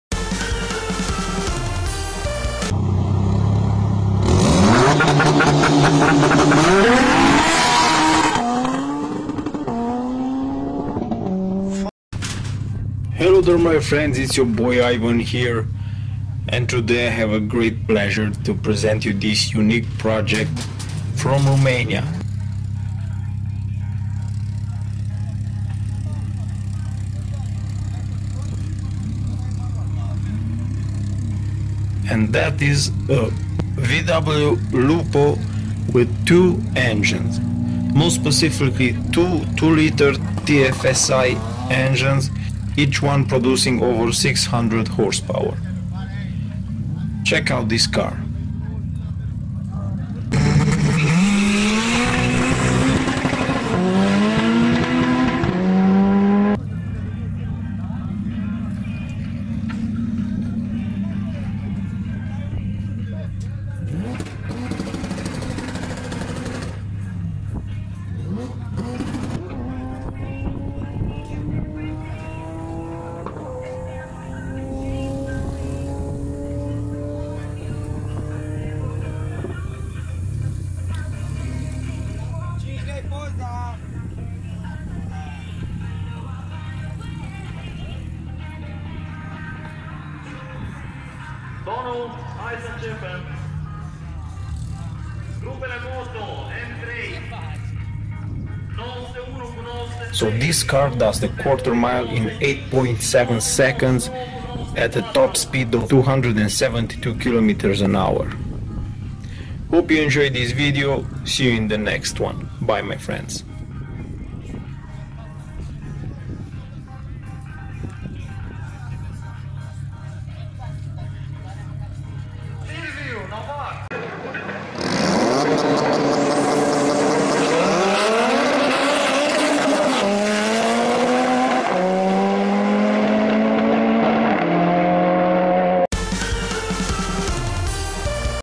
This is the FASTEST VW in the World - Faster than a Bugatti Veyron
Welcome to Ivan, the Car Whiesperer.

Hope you will find my videos helpful.

This is the FASTEST VW in the World. 2 x 600 HP ENGINE

Come back to the channel for more car reviews and fixing tips.